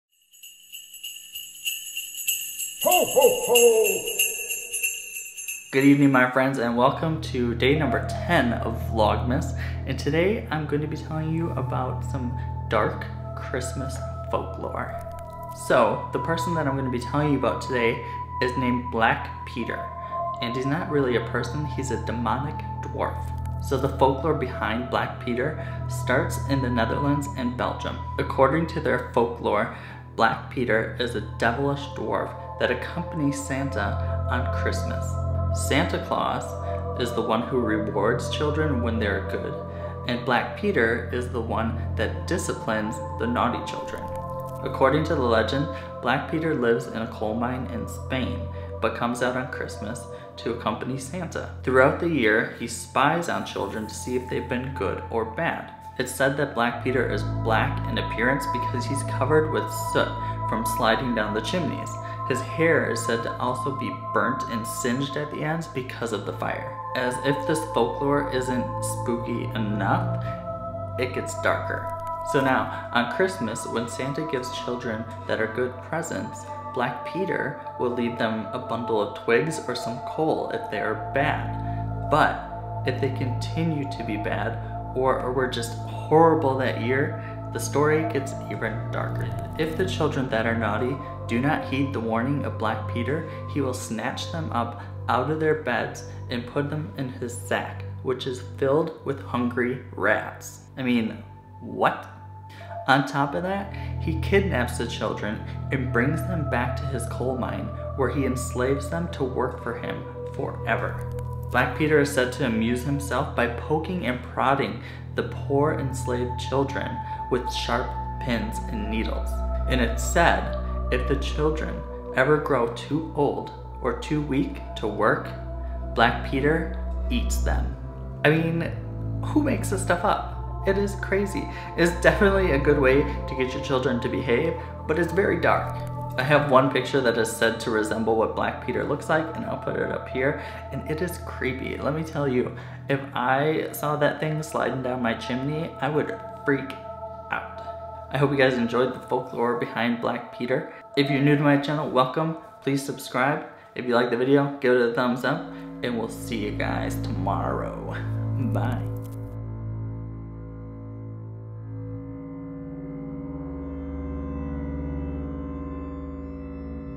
Vlogmas | Day 10 | Black Peter | MichaelScot
Hello Friends!

Welcome to day 10 of Vlogmas!  Today I wanted to tell you about a dark Christmas legend!  This video is on Black Peter!  I hope you enjoy it!

Music